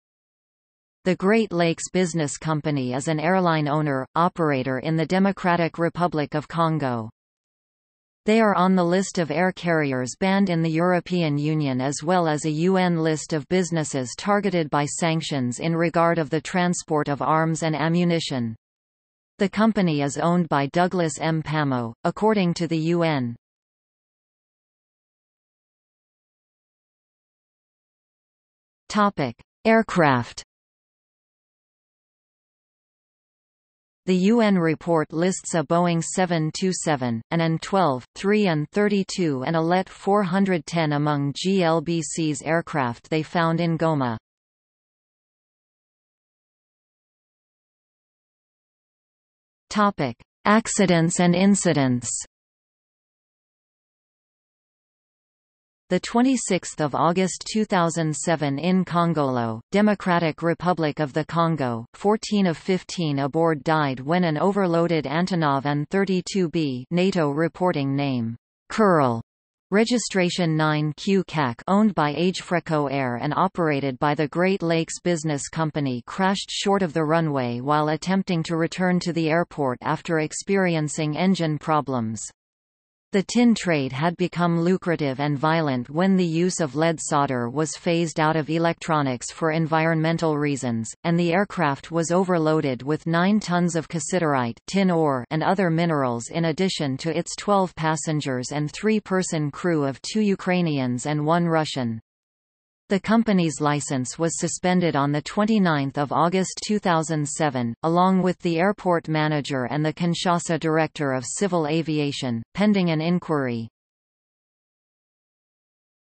Great Lakes Business Company | Wikipedia audio article
00:00:25 Aircraft
00:00:46 Accidents and Incidents

- Socrates

SUMMARY
The Great Lakes Business Company is an airline owner/operator in the Democratic Republic of Congo.
They are on the List of air carriers banned in the European Union as well as a UN list of businesses targeted by sanctions in regard of the transport of arms and ammunition.   The company is owned by Douglas Mpamo, according to the UN.